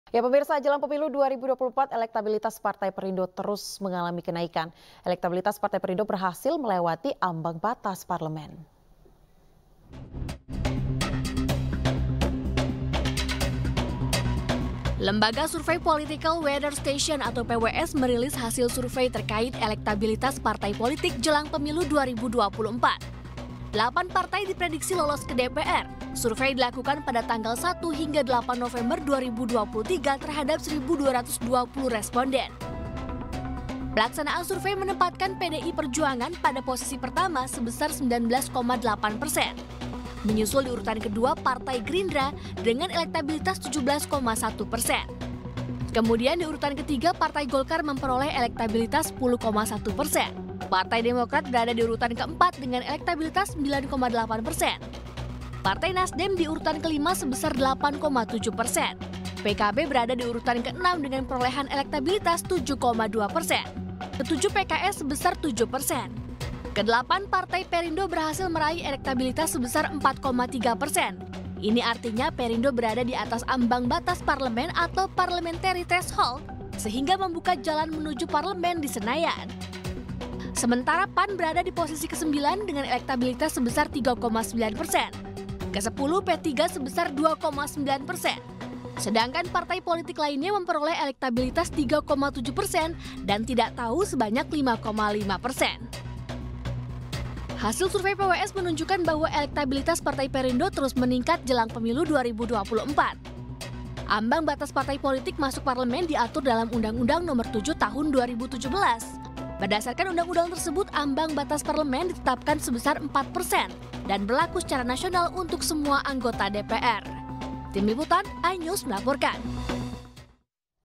Elektabilitas Partai Perindo Lewati Ambang Batas parlemen
Elektabilitas atau tingkat keterpilihan Partai Perindo saat ini mencapai 4,3%. Hal ini berdasarkan hasil survei yang dilakukan lembaga Survei PWS.